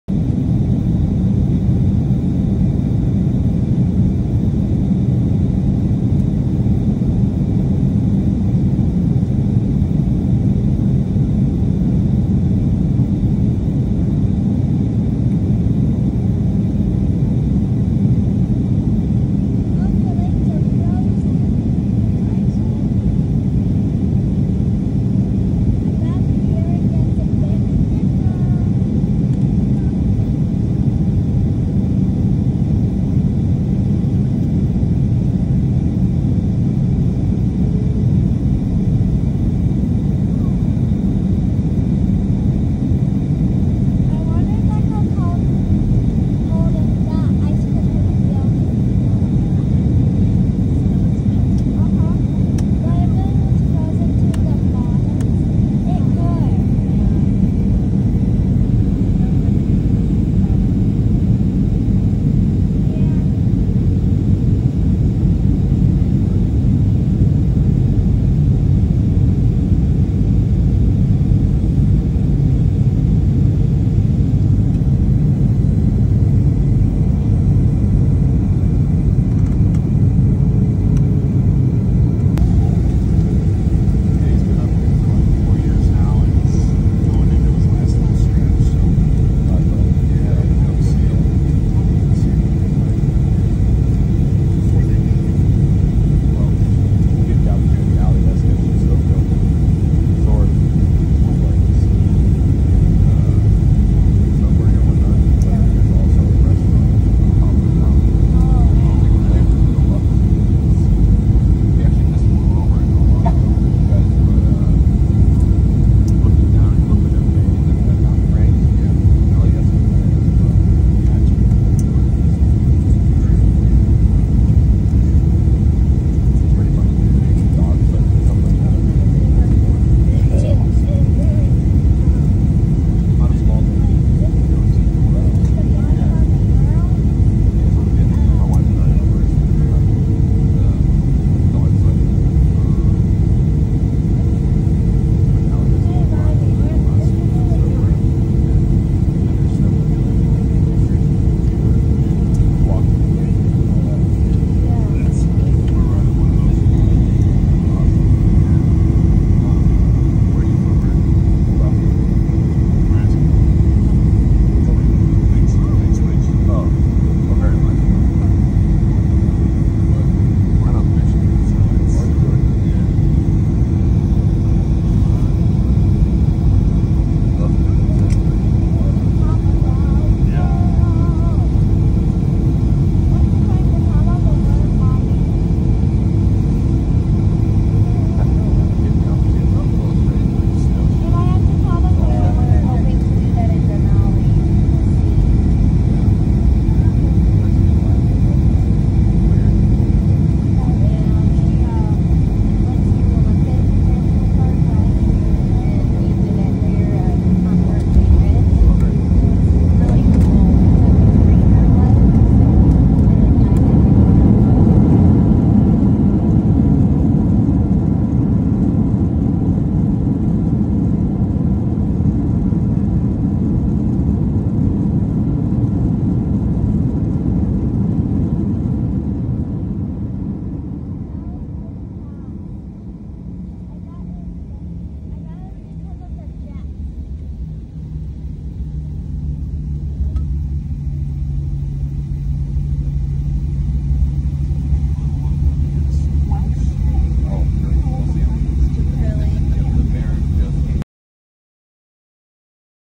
April 2024. Delta flight from Seattle to Anchorage Ted Stevens airport.
This flight captured the final approach into the Ted Stevens airport in Anchorage Alaska. You can see the tundra covered in ice and snow and some views of ice capped mountains as we approach and land. Comment and rate as I will be posting more of my flight experiences. Thanks